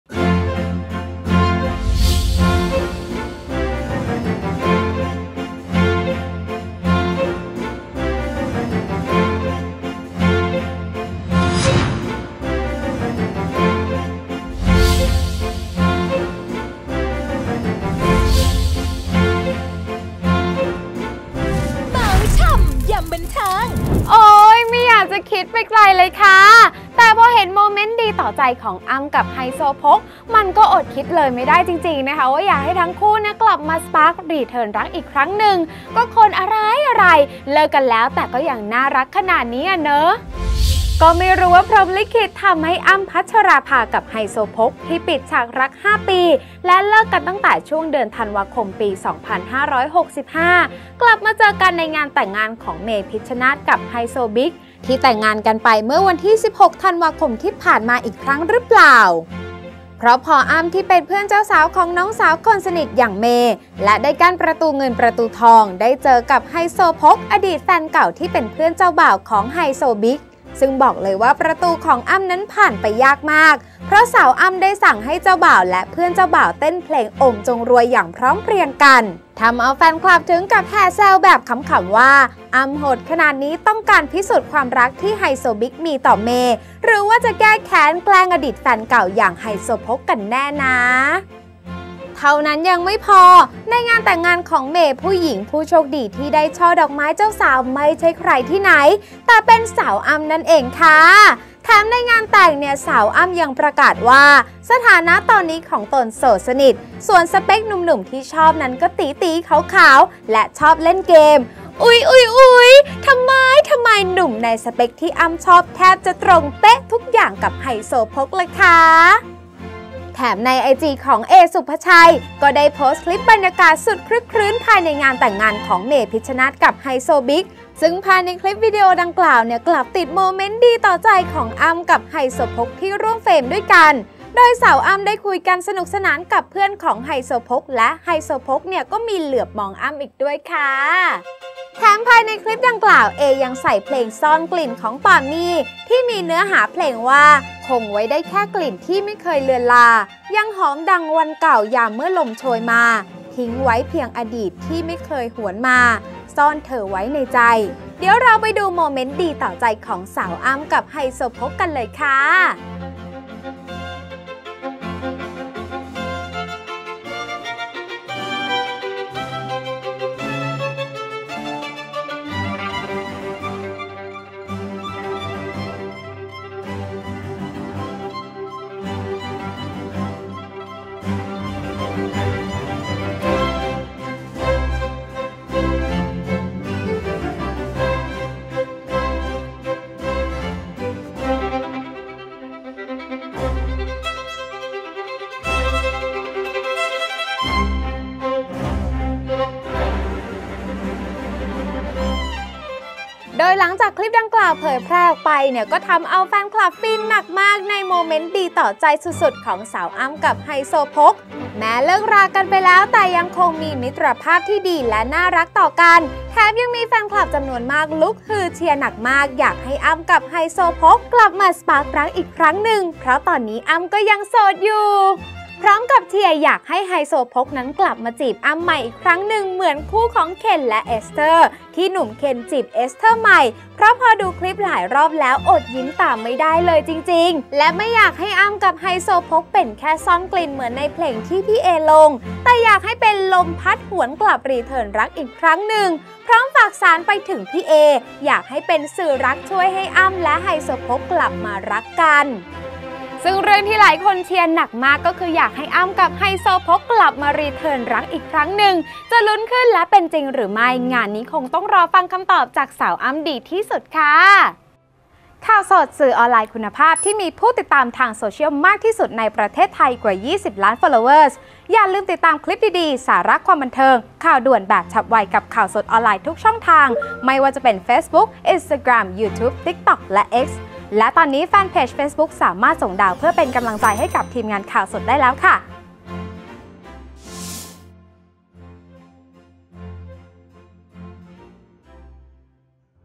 โมเมนต์ดีต่อใจ! ‘อั้ม-ไฮโซพก’ ลุ้นรีเทิร์นรัก! เห็นคลิป ‘เอ ศุภชัย’ แห่เชียร์สนั่น: Khaosod - ข่าวสด
โอ๊ยไม่อยากคิดไปไกลเลยค่า แต่พอเห็นโมเมนต์ดีต่อใจของ “อั้ม“ กับ ”ไฮโซพก“ มันอดคิดเลยไม่ได้จริงๆว่า อยากให้ทั้งคู่กลับมาสปาร์ครักรีเทิร์นรักอีกึครั้งหนึ่ง ก็คนอะไรเลิกกันแล้วแต่ยังน่ารักขนาดนี้อ่าเนอะ